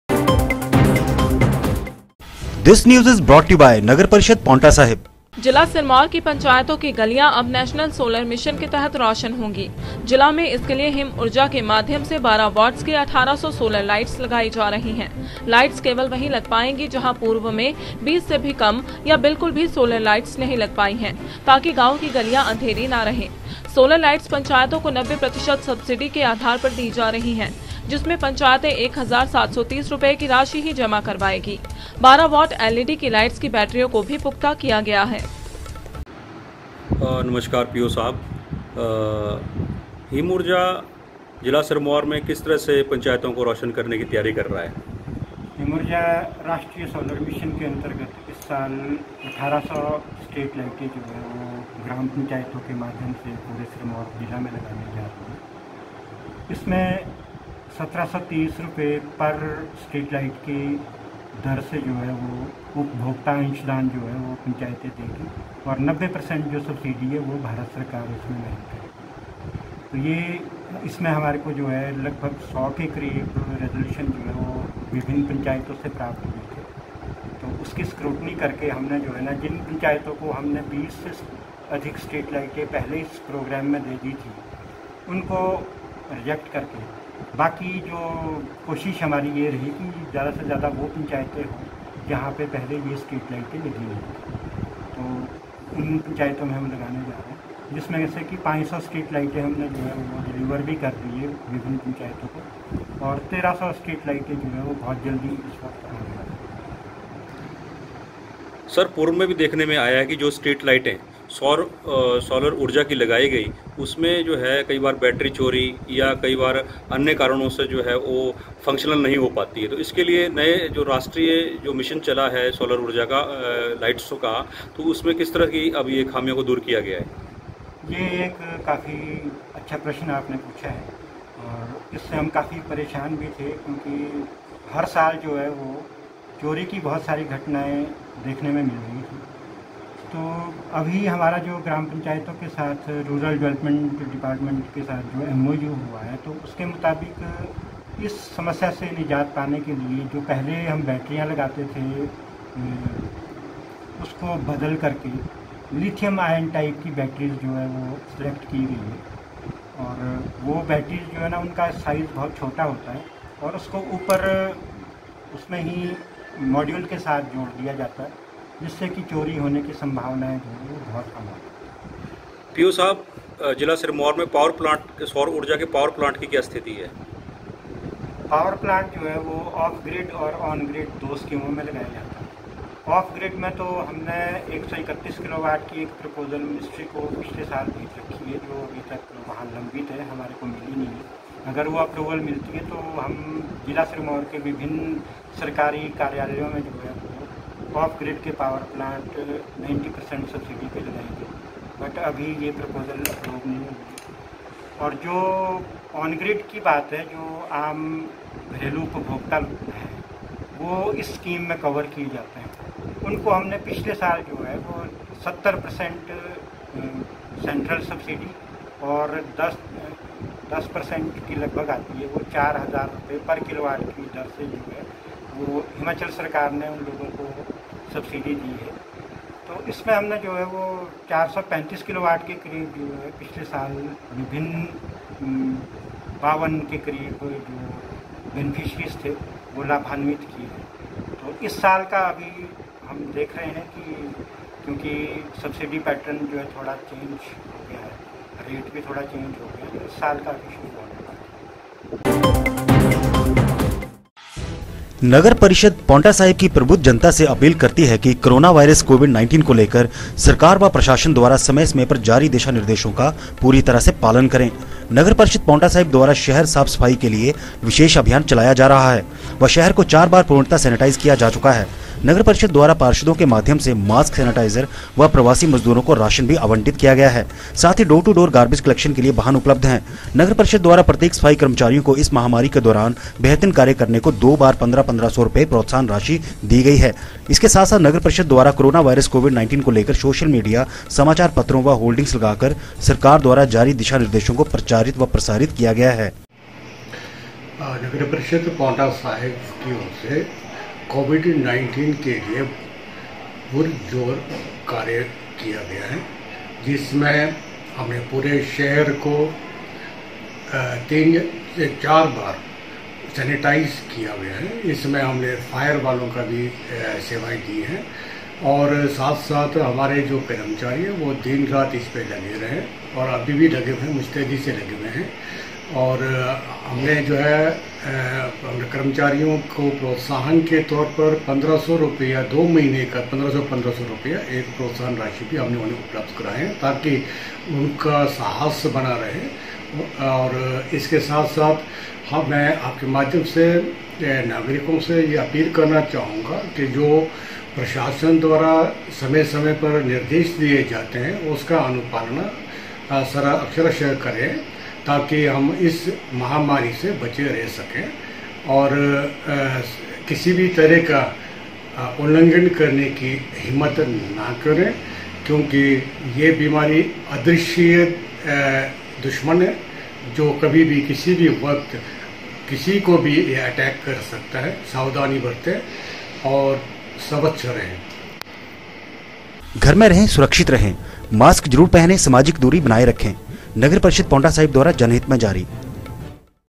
सोलर लाइट्स से रोशन होंगी सिरमौर की गलियां, 90 फीसदी सबसिडी भी मिलेगी
सोलर लाइट्स से रोशन होंगी सिरमौर की गलियां, 90 फीसदी सबसिडी भी मिलेगी, हिम ऊर्जा की मुहिम लाई रंग